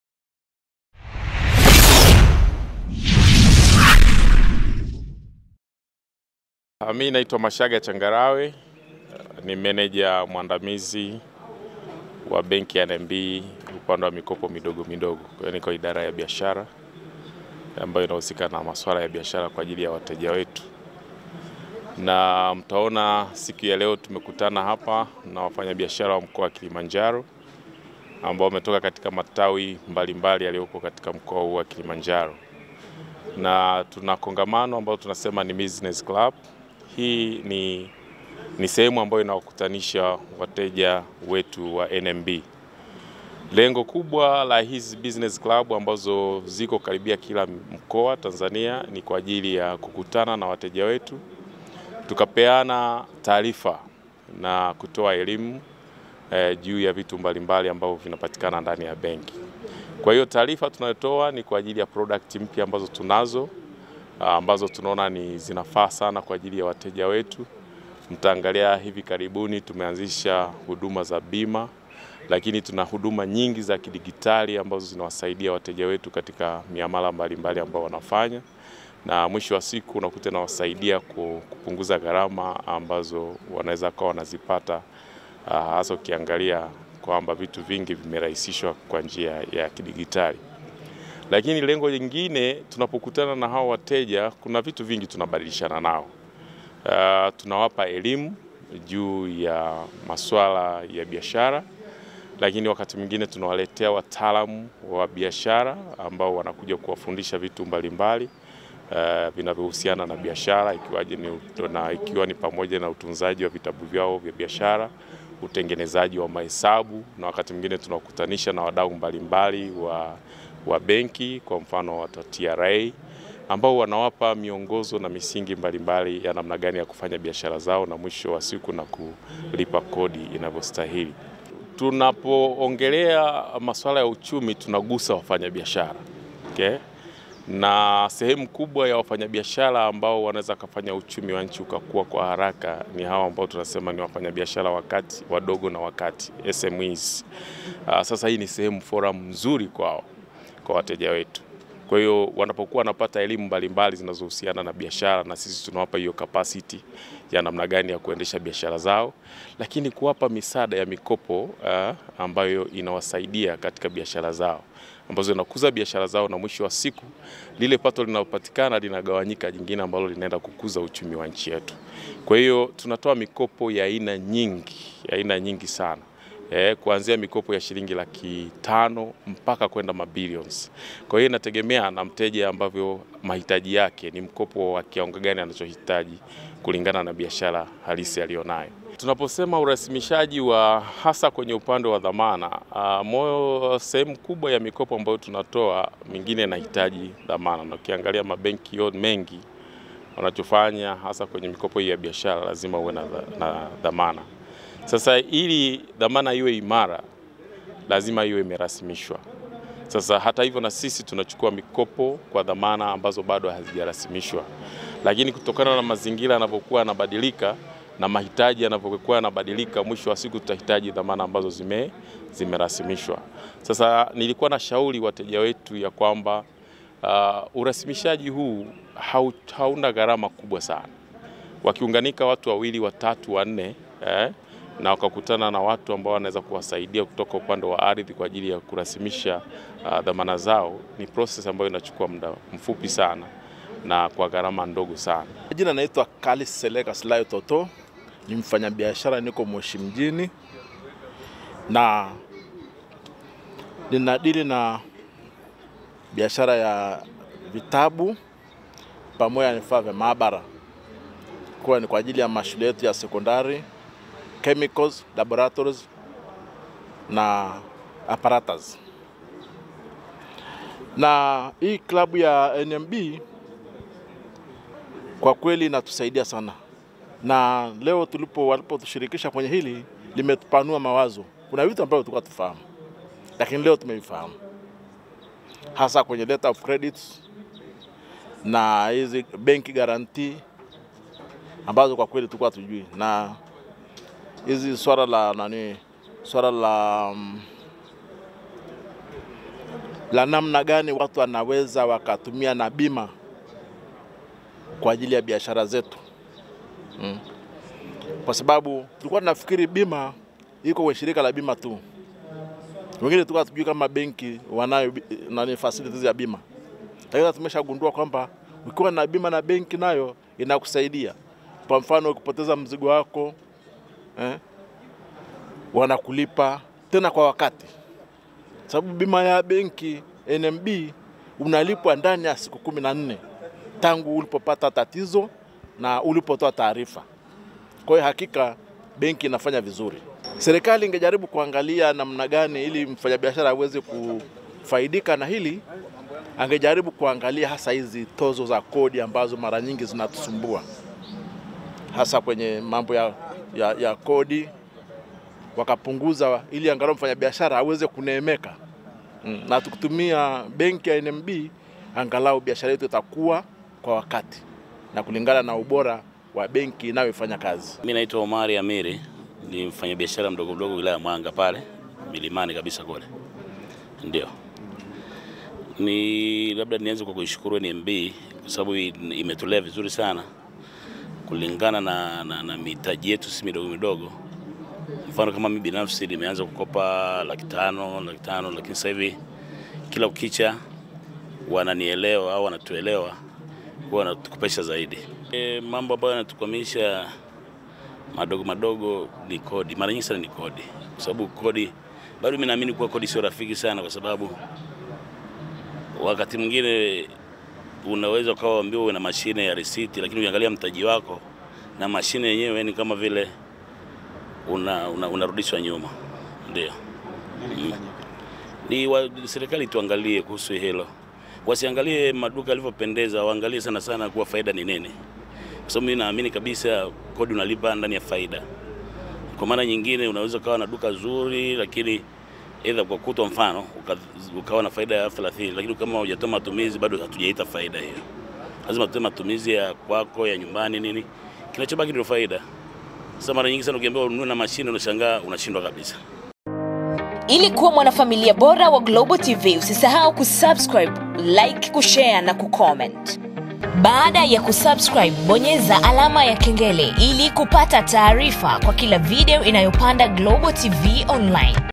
NMB WAZINDUA HUDUMA MPYA/YAWAKUTANISHA WAFANYABIASHARA "WACHUKUE MIKOPO WAKAANZE BIASHARA"
NMB WAZINDUA HUDUMA MPYA/YAWAKUTANISHA WAFANYABIASHARA "WACHUKUE MIKOPO"